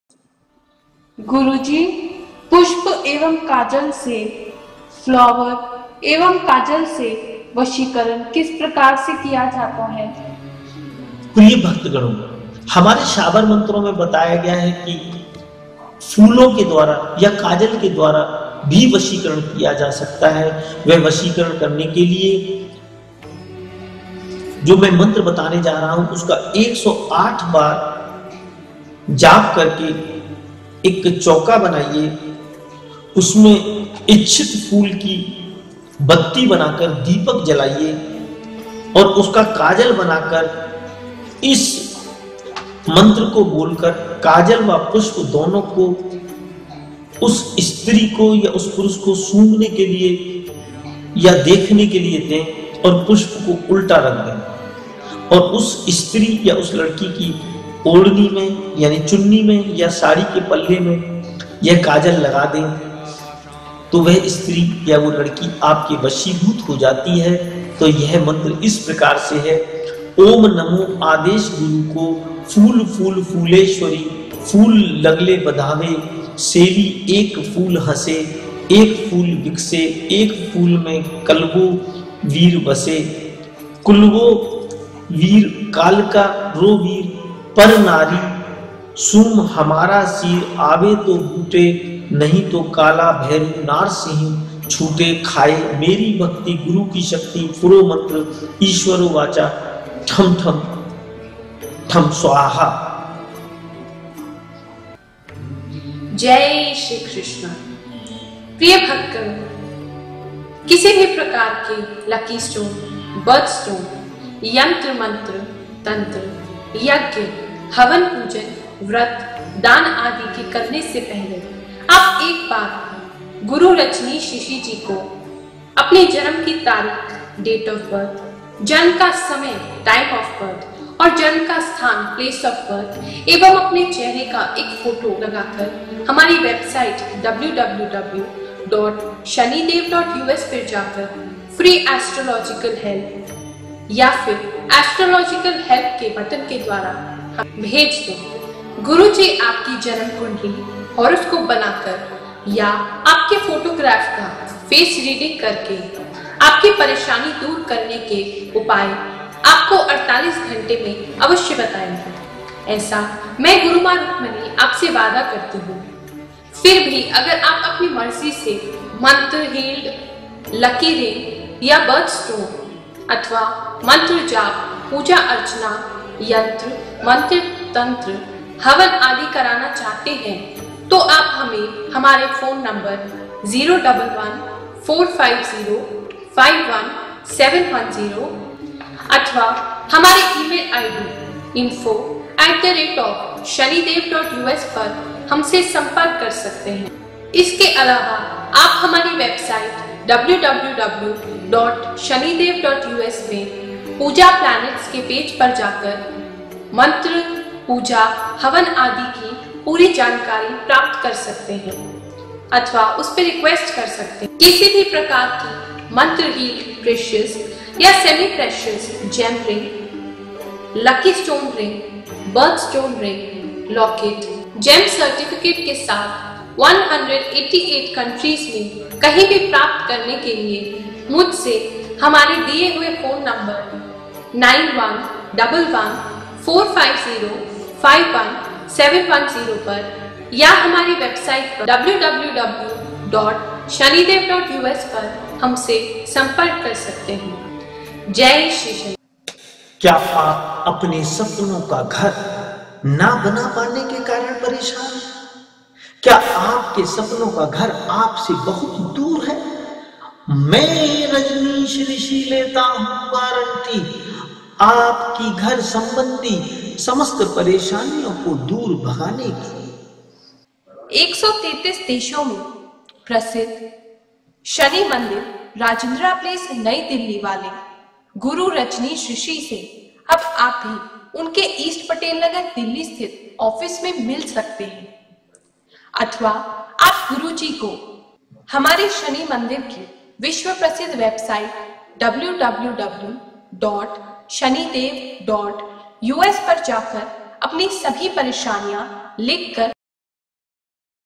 Get Your Love Back Mantra by Param Pujya Guru Rajneesh Rishi Ji
Get Your Love Back Mantra  by Param Pujya Guru Rajneesh Rishi Ji, World Famous Spiritual Guru in

133 Countries, Shani Mandir Wale, New Delhi. India.

Note : Please note that this Information given in the video should not be used with any badintentions and should be always use in our Guidance.

Vashikaran Mantra is used to cast love spell on someone whom you want to make you love. Shabar mantras are very simple and easy to use. The most powerful feature of Shabar mantras is that they are auto-powered and highly effective at once. These are not difficult as that of Classical mantras. Whenever you want to use a mantra, it is necessary this to attain power but in shabar mantras it is not so. They are in simple and local Indian languages and can be used by anyone. They are user-friendly.  Shabar Mantras are found in every language and most of the problems can be solved using them. These mantras are found in two types. The first, which are not easy to understand because words used in them are difficult and the second one, which are easily understood as their words are simple to understand.